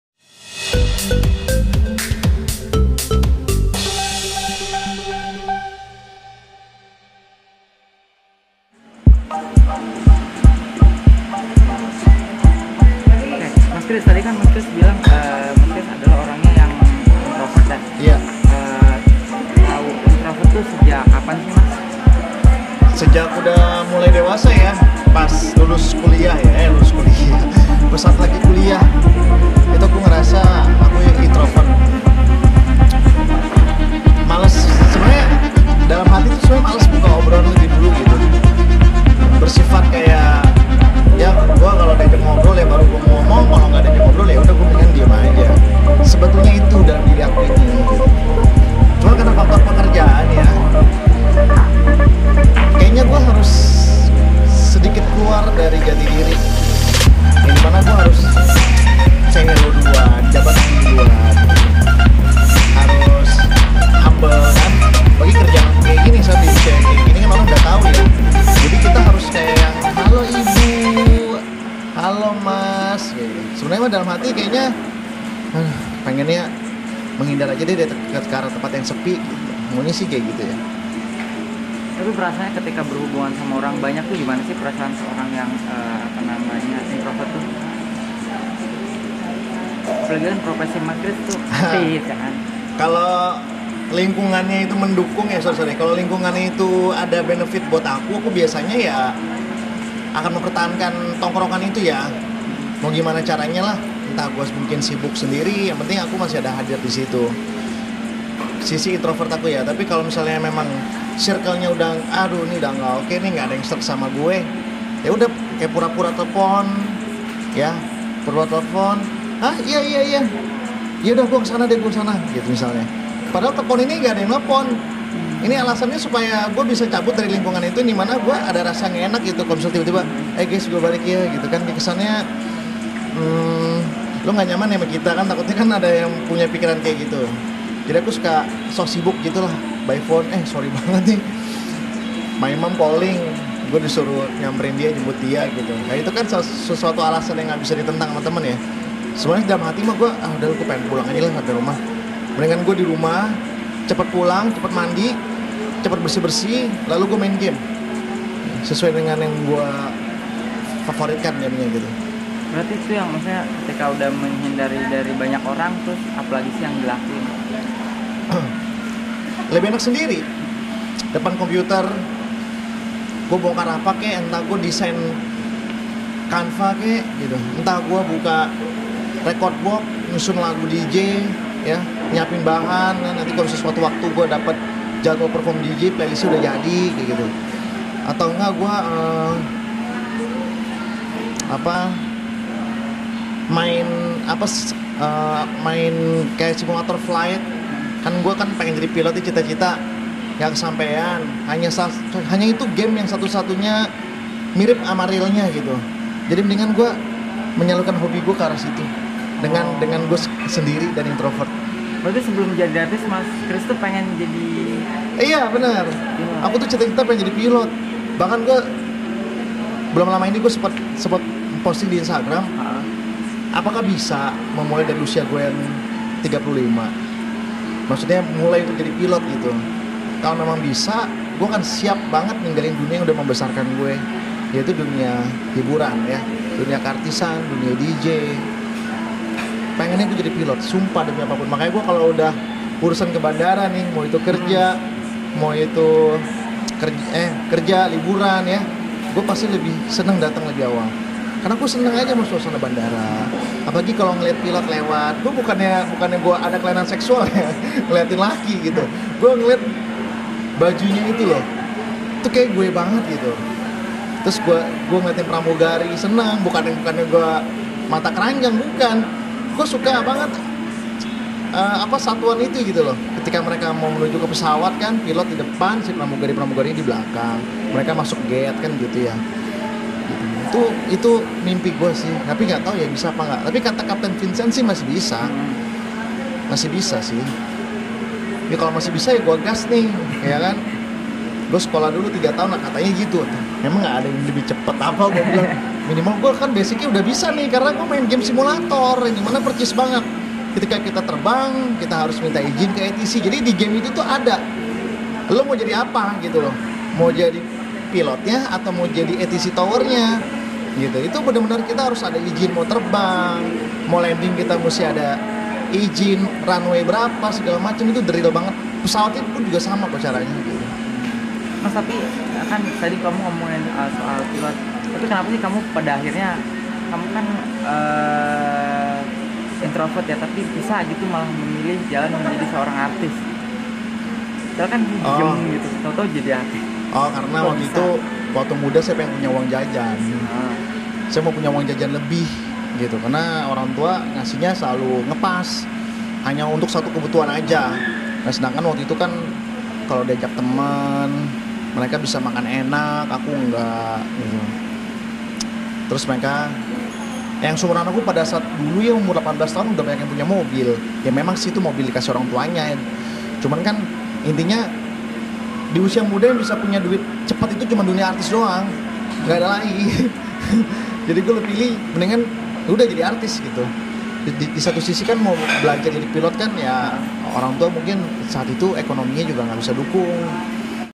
TERNYATA KRISS HATTA ORANG INTROVERT DAN PERNAH LAKUKAN INI ⁉️
Orang introvert itu tidak suka dengan keramaian yang membuatnya dirinya menyendiri, Kriss Hatta mempunyai hal itu dimana dia pernah kejadian ketika berkumpul dengan teman tapi saat dirinya sudah tidak nyaman dengan sekitar Kriss Hatta sampai pura-pura telephone seseorang untuk pergi dari tempat itu.